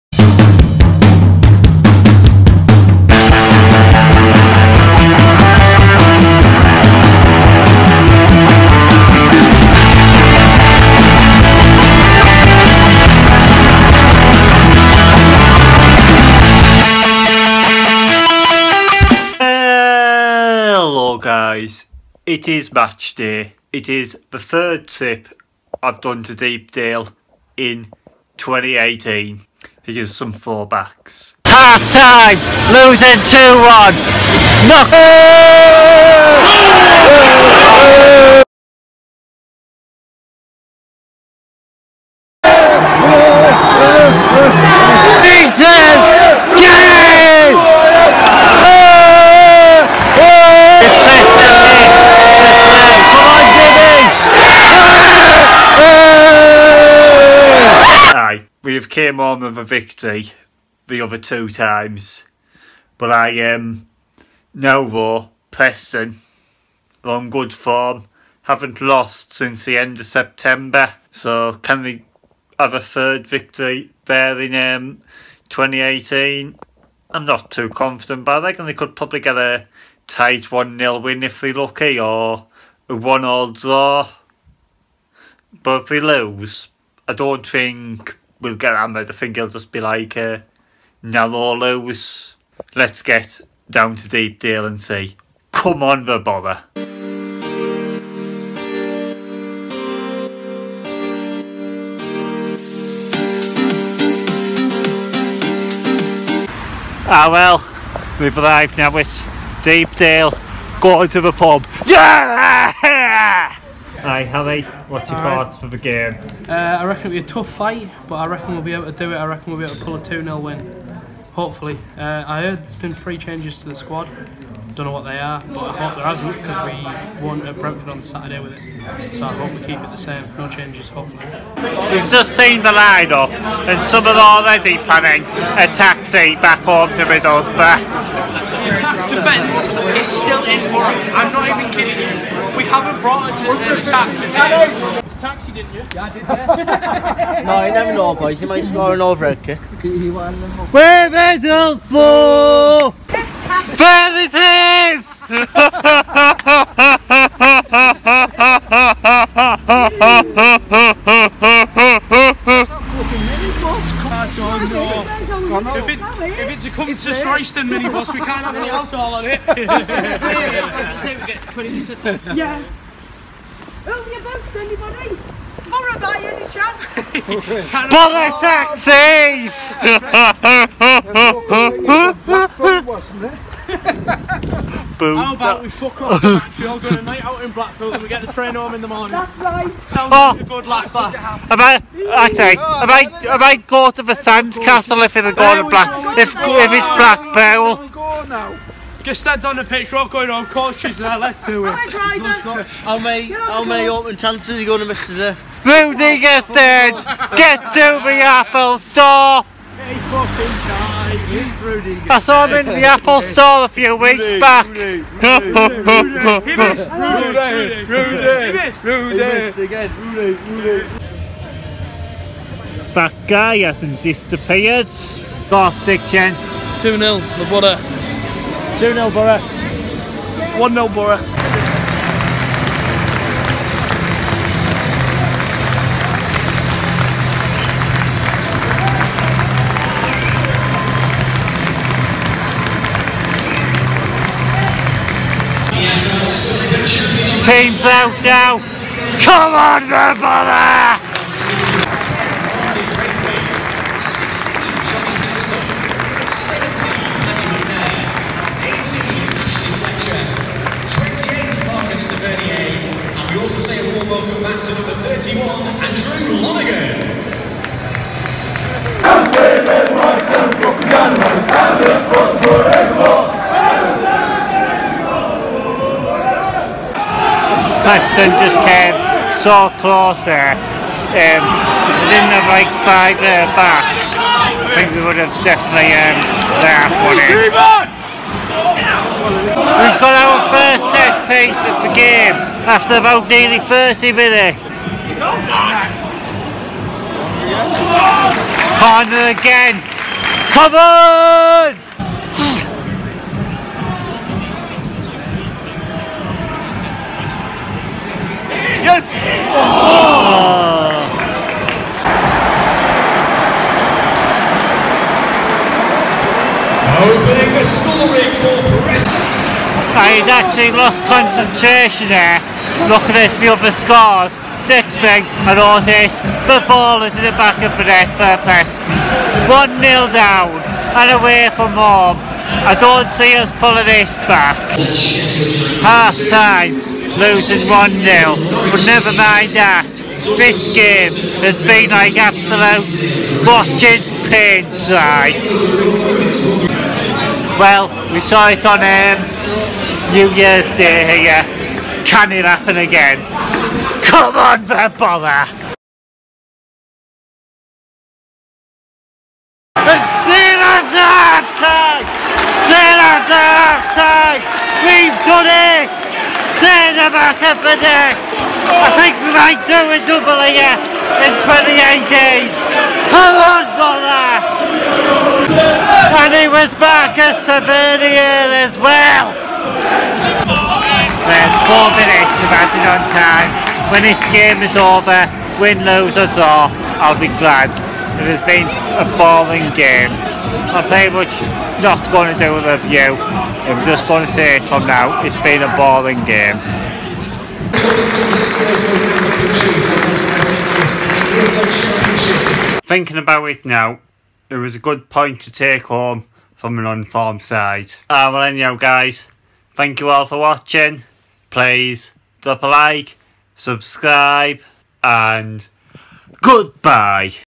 And It's Marcus Tavernierrrrr Again - Middlesbrough vs Preston North End
Preston North End 1 Middlesbrough 1.